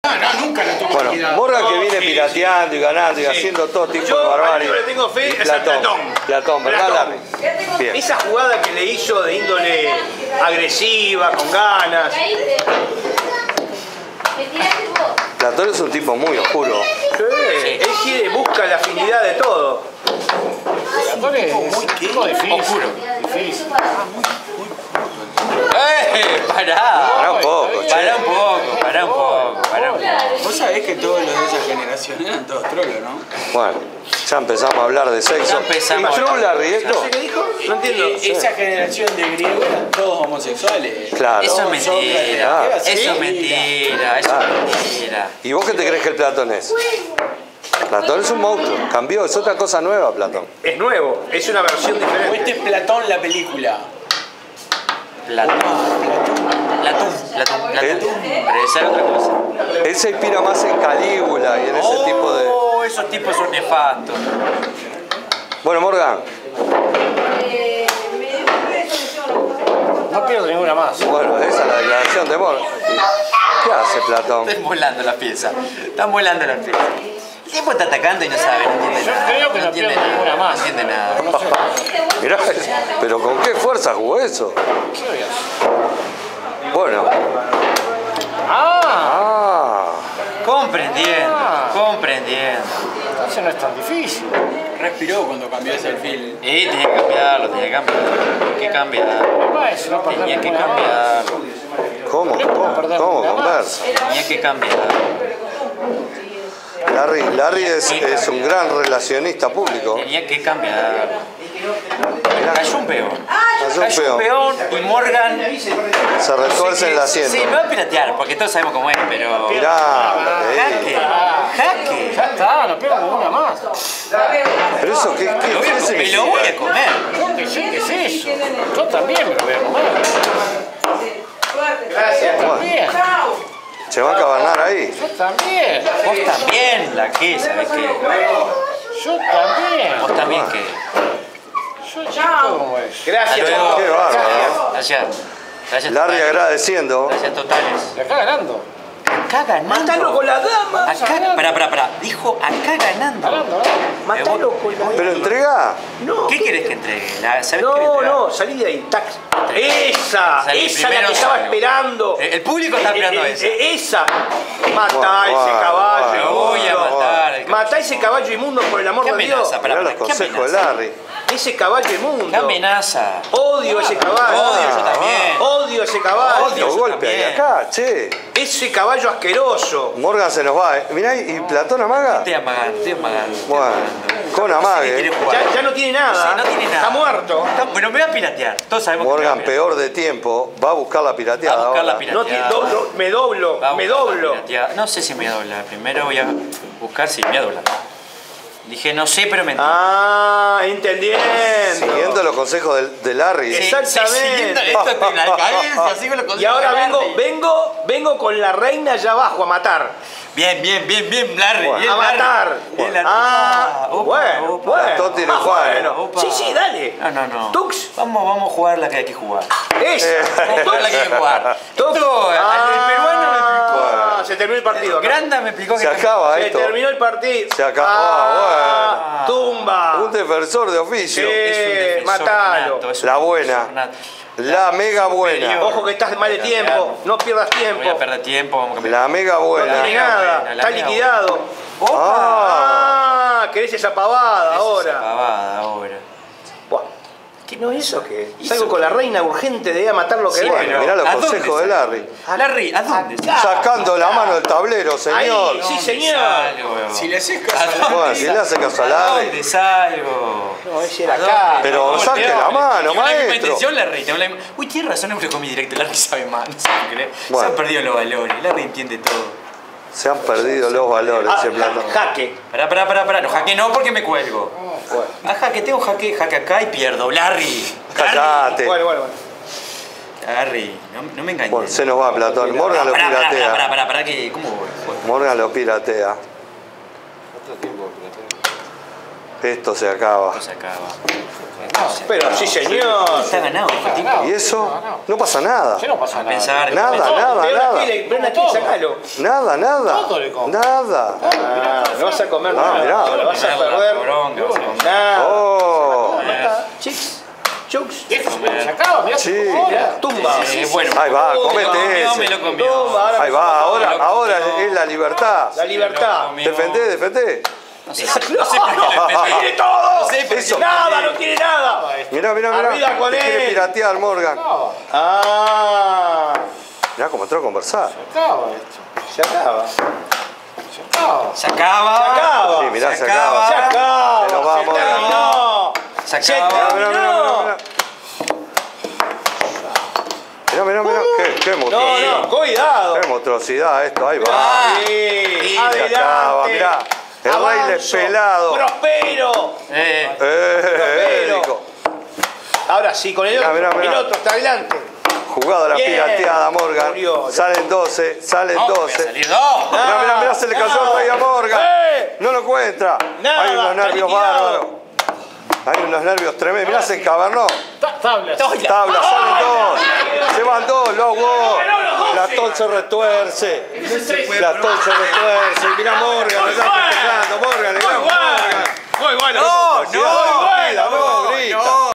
ajedrez callejero (Morgan-Platón) (II)
NUEVO ¡¡¡ Amigos del ajedrez bala a 3 min, Sres revancha platón venía dispuesto a todo en la apertura enrroques opuestos, Platón al longo, Morgan ya se le va arriba con todo manda la dama,hay cambios Platón se defiende y ataca, Y Morgan le entrega un caballo para abrirlo, Platón clavado resiste, se desclava, el ataque se diluye y ya se olfatea la pirateada, Platón canbia torres y en un final de locos Morgan sin nada resiste y salen dos por tiempo, Expreso,Pocitos,Montevideo,Uruguay.